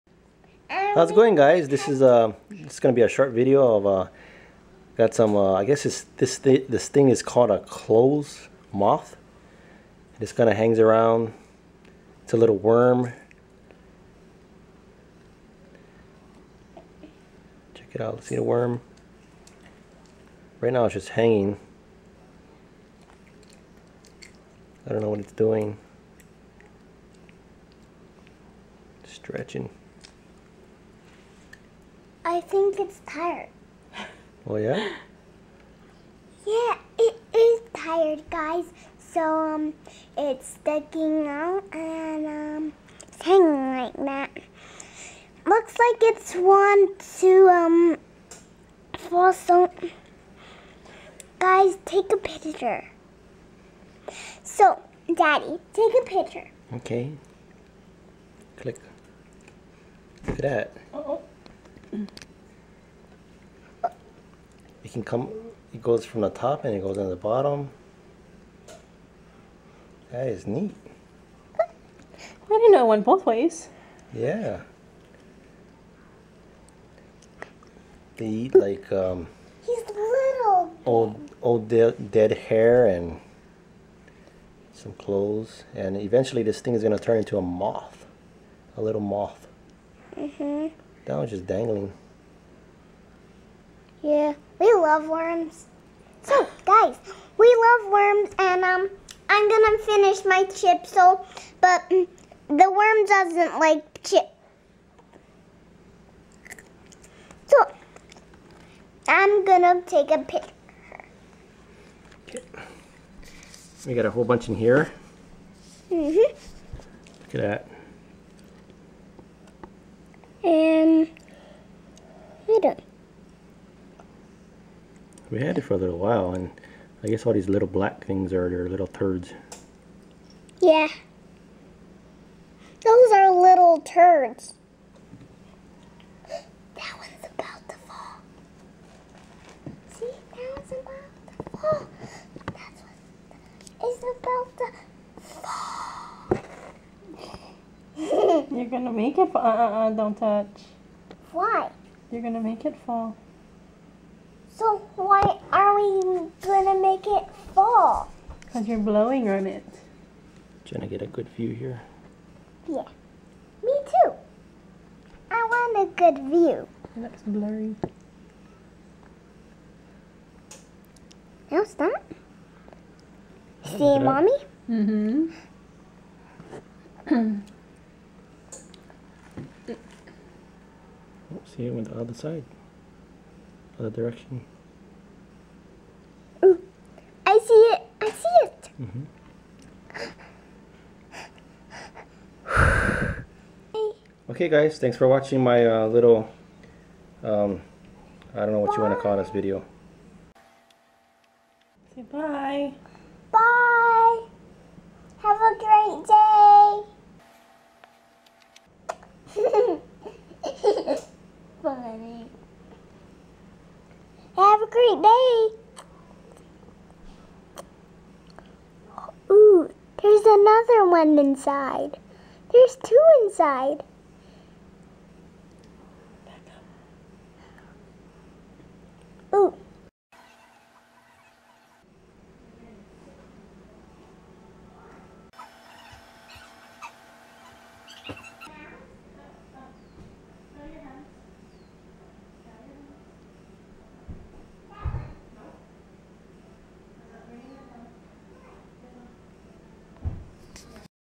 Clothes Moth close up
Hey guys just sharing a short video on some Clothes Moth Larvae.
They make small homes made of things they feed on. I think these moths are responsible for the little holes in our clothes. The larvae eventually turn into Moths.